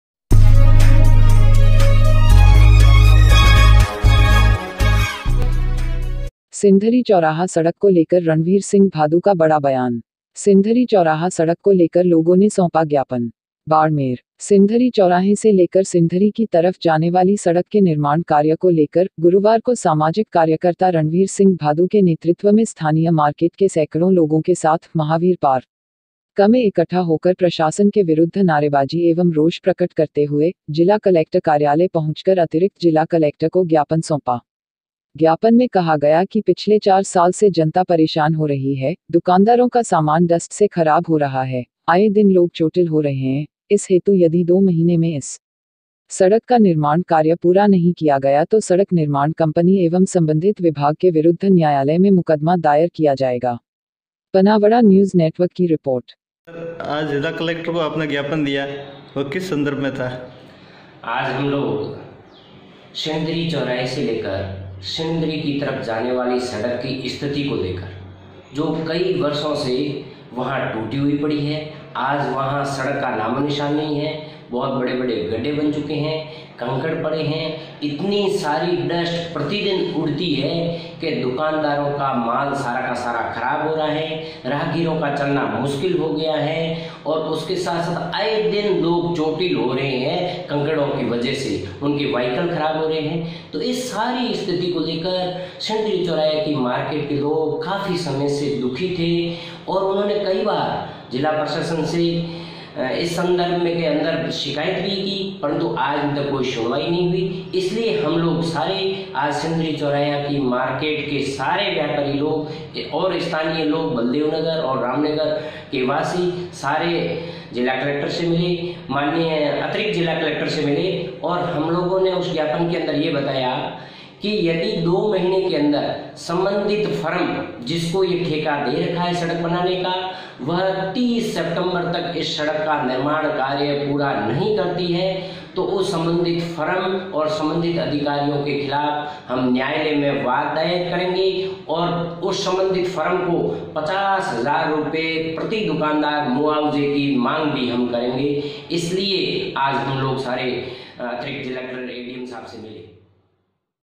सिणधरी चौराहा सड़क को लेकर रणवीर सिंह भादू का बड़ा बयान। Panawara TV
सिणधरी चौराहा सड़क को लेकर लोगो ने सौंपा ज्ञापन।
बाड़मेर। सिणधरी चौराहे से लेकर सिणधरी की तरफ जाने वाली सड़क के निर्माण कार्य को लेकर, गुरुवार को सामाजिक कार्यकर्ता रणवीर सिंह भादू के नेतृत्व में स्थानीय मार्केट के सैकड़ों लोगों के साथ, महावीर पार्क में इकठा होकर प्रशासन के विरुद्ध नारेबाजी एवं रोष प्रकट करते हुए, जिला कलेक्टर कार्यालय पहुंचकर अतिरिक्त जिला कलेक्टर को ज्ञापन सौंपा। ज्ञापन में कहा गया कि पिछले चार साल से जनता परेशान हो रही है, दुकानदारों का सामान डस्ट से खराब हो रहा है, आये दिन लोग चोटिल हो रहे हैं, इस हेतु यदि दो महीने में इस सड़क का निर्माण कार्य पूरा नहीं किया गया तो सड़क निर्माण कंपनी एवं सम्बन्धित विभाग के विरुद्ध न्यायालय में मुकदमा दायर किया जायेगा।
पनावड़ा न्यूज़ नेटवर्क की रिपोर्ट।